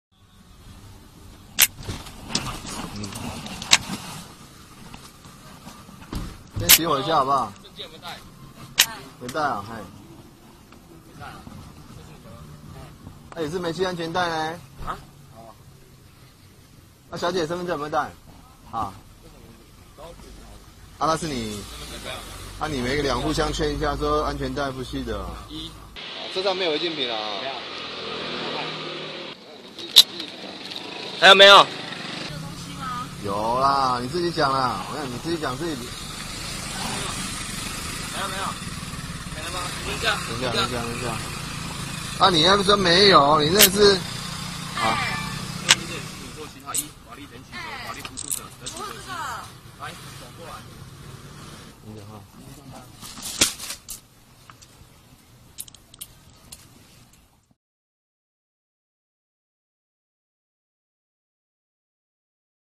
油漆工車內藏毒被抓 女友氣到怒巴他頭
27歲周姓油漆工開車載女友上班，因忘繫安全帶被高市保大特警攔查，當場搜出潮牌「Aape」圖騰的毒咖啡，重量多達200多公克，張姓女友見男友碰毒，相當錯愕，氣到拿袋子狠甩男友，怒斥「怎碰這種東西」，警方今天訊後將周男送辦，仍對張女採尿送驗。